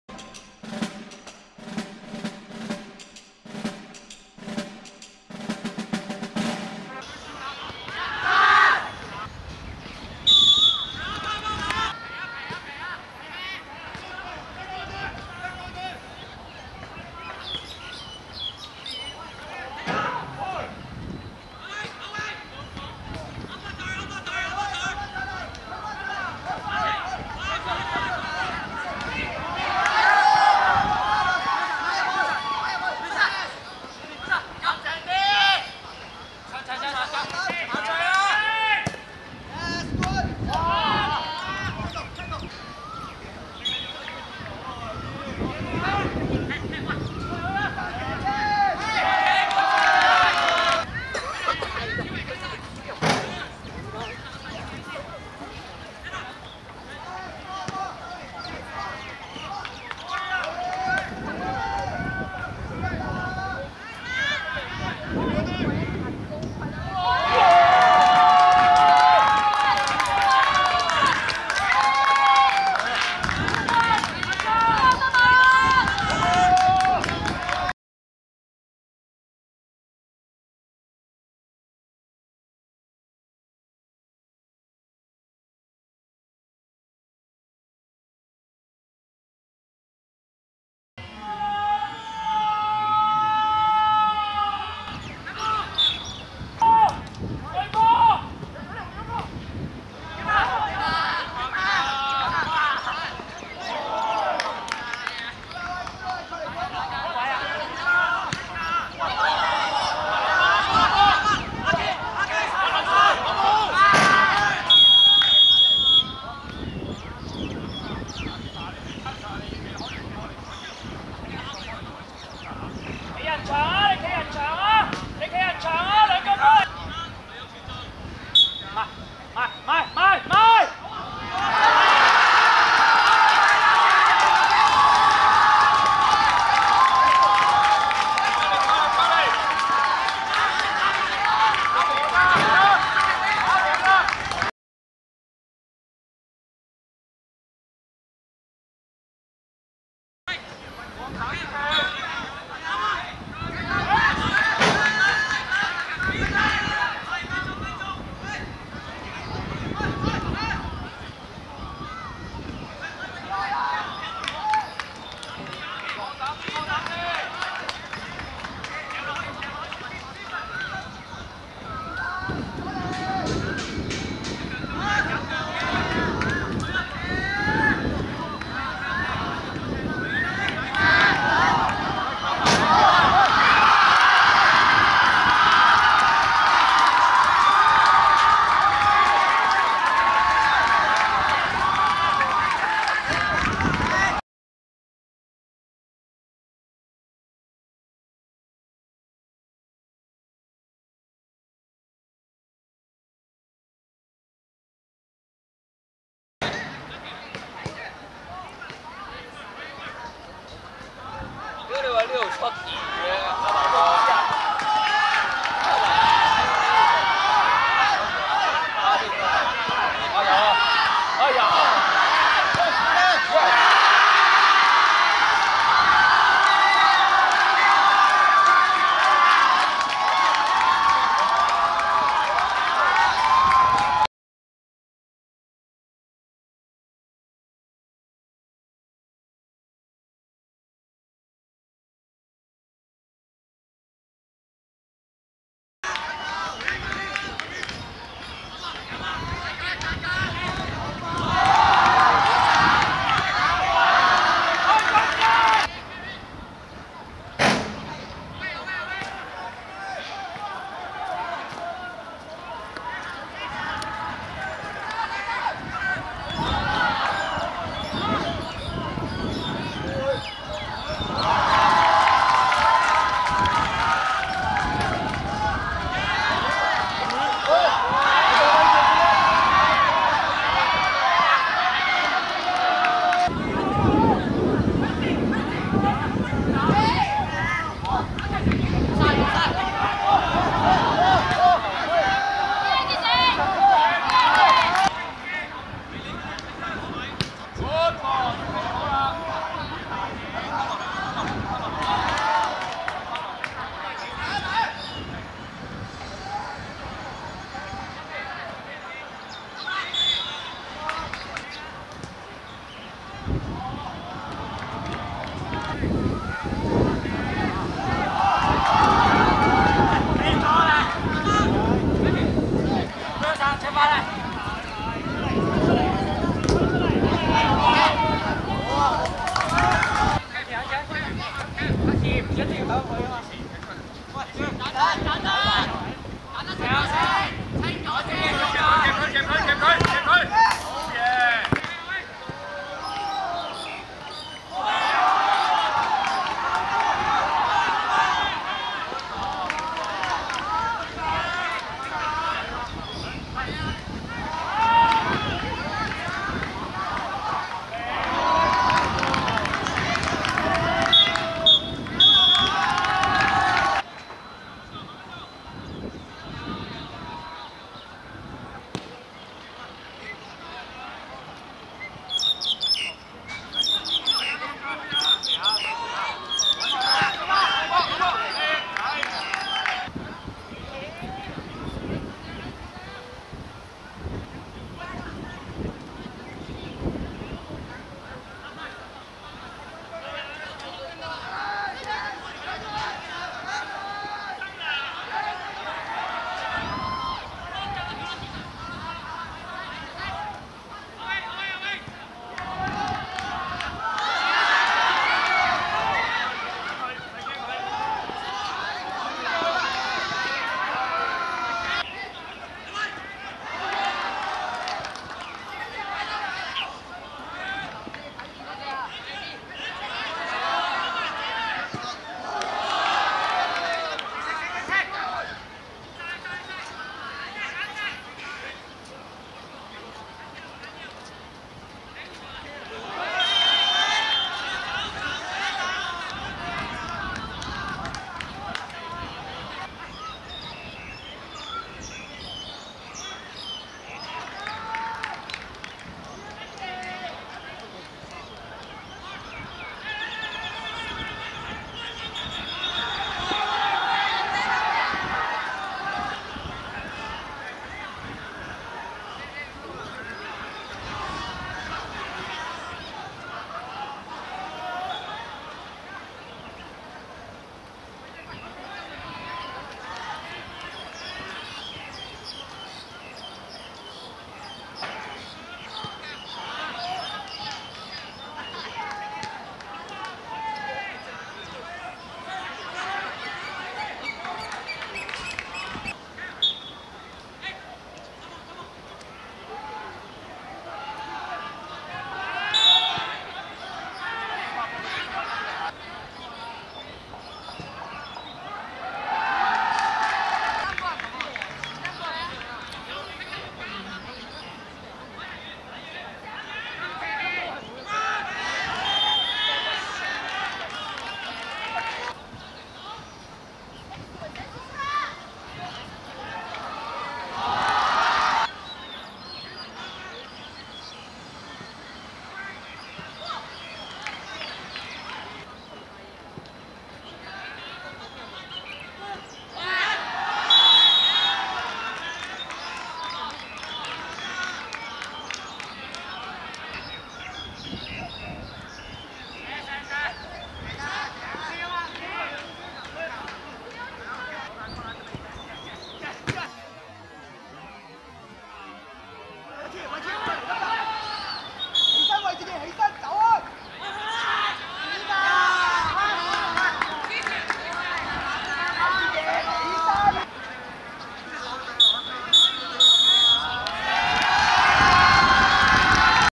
新民vs瑪利諾神父(2023.3.18.D3K2學界足球乙組決賽)精華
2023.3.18.九龍仔3號塲 14:30
新民(淺藍) 1 : 3 瑪利諾神父(深藍)